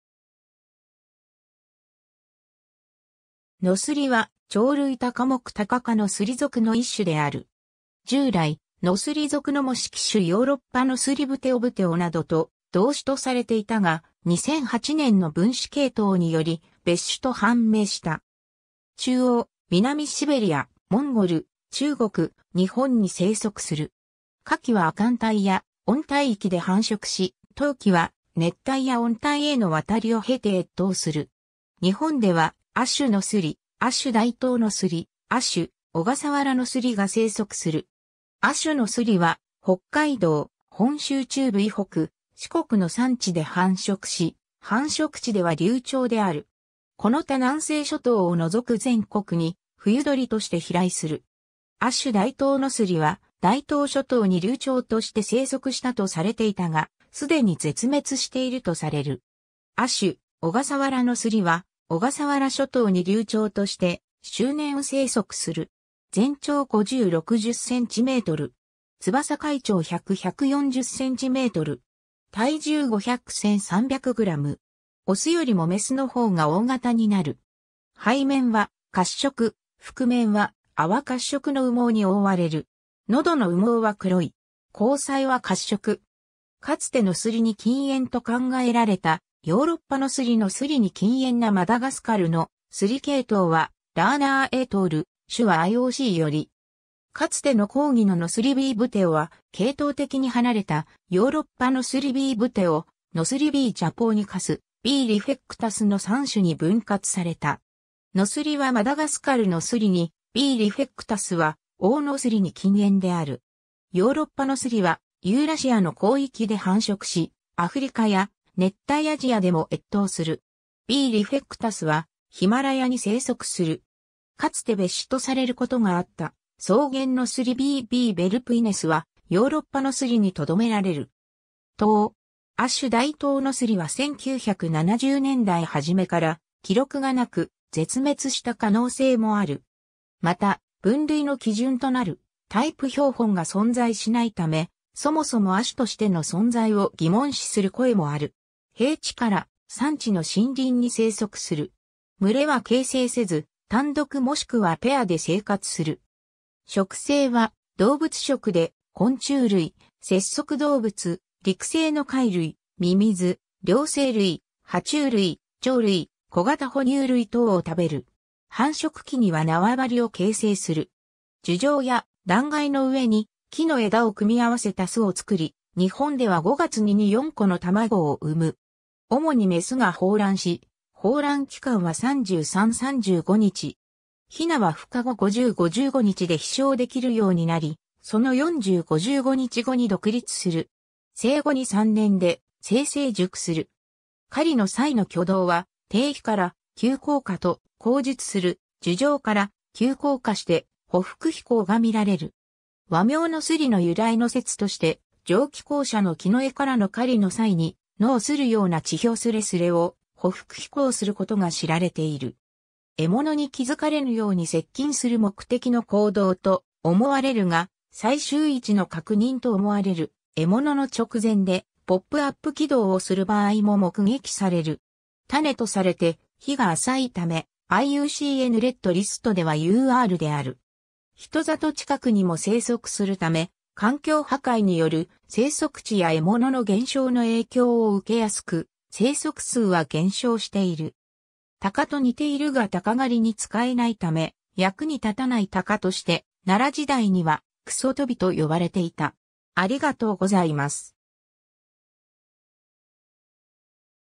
ノスリ
絶滅_(環境省レッドリスト)
ノスリ（鵟、学名 Buteo japonicus）は、鳥類タカ目タカ科ノスリ属の1種である。
 従来、ノスリ属の模式種ヨーロッパノスリ Buteo buteo などと同種とされていたが、2008年の分子系統により別種と判明した。
 中央・南シベリア、モンゴル、中国、日本に棲息する。
夏季は亜寒帯や温帯域で繁殖し、冬季は熱帯や温帯への渡りを経て越冬する。
 日本では亜種ノスリ、亜種ダイトウノスリ、亜種オガサワラノスリが生息する。
亜種ノスリは、北海道、本州中部以北、四国の山地で繁殖し、繁殖地では留鳥である。
この他南西諸島を除く全国に冬鳥として飛来する。
亜種ダイトウノスリは大東諸島に留鳥として生息したとされていたが、すでに絶滅しているとされる。
亜種オガサワラノスリは小笠原諸島に留鳥として周年生息する。
 全長50–60cm。
翼開長100–140cm。
体重500–1300g。
オスよりもメスの方が大型になる。
背面は褐色、腹面は淡褐色の羽毛に覆われる。
喉の羽毛は黒い。
 虹彩は褐色。
 かつてノスリに近縁と考えられたヨーロッパノスリ ノスリに近縁なマダガスカルノスリ 系統は Lerner et al. (2008)、種はIOCより。
 かつての広義のノスリ B. buteo は、系統的に離れたヨーロッパノスリ B. buteo、ノスリ B. japonicus、B. refectus の3種に分割された。
ノスリはマダガスカルノスリに、B. refectus はオオノスリに近縁である。
 ヨーロッパノスリはユーラシアの広域で繁殖し、アフリカや熱帯アジアでも越冬する。
B. refectus はヒマラヤに棲息する。
 かつて別種とされることがあったソウゲンノスリ B. b. vulpinus は、ヨーロッパノスリにとどめられる。
 等 ? 亜種ダイトウノスリは1970年代初めから記録がなく、絶滅した可能性もある。
また、分類の基準となるタイプ標本が存在しないため、そもそも亜種としての存在を疑問視する声もある。
 平地から山地の森林に生息する。
群れは形成せず、単独もしくはペアで生活する。
 食性は動物食で、昆虫類、節足動物、陸棲の貝類、ミミズ、両生類、爬虫類、鳥類、小型哺乳類等を食べる。
 繁殖期には縄張りを形成する。
樹上や断崖の上に木の枝を組み合わせた巣を作り、日本では5月に2–4個の卵を産む。
 主にメスが抱卵（雌雄とも抱卵することもある）し、抱卵期間は33–35日。
雛は孵化後50–55日で飛翔できるようになり、その40–55日後に独立する。
生後2–3年で性成熟する。
 狩りの際の挙動は、「停飛から急降下」と、後述する「樹上から急降下して匍匐飛行」が見られる。
 和名ノスリの由来の説として、上記後者の樹上からの狩りの際に、野を擦るような地表すれすれを匍匐飛行する（野擦）ことが知られている。
 獲物に気づかれぬように接近する目的の行動と思われるが、最終位置の確認と思われる「獲物の直前でポップアップ機動」をする場合も目撃される。
 種とされて日が浅いため、IUCN Red List ではUR (Under Review ※調査中) である。
 人里近くにも生息するため、環境破壊による生息地や獲物の減少の影響を受けやすく、生息数は減少している。
 鷹と似ているが鷹狩に使えないため、役に立たない鷹として、奈良時代には「くそとび」と呼ばれていた。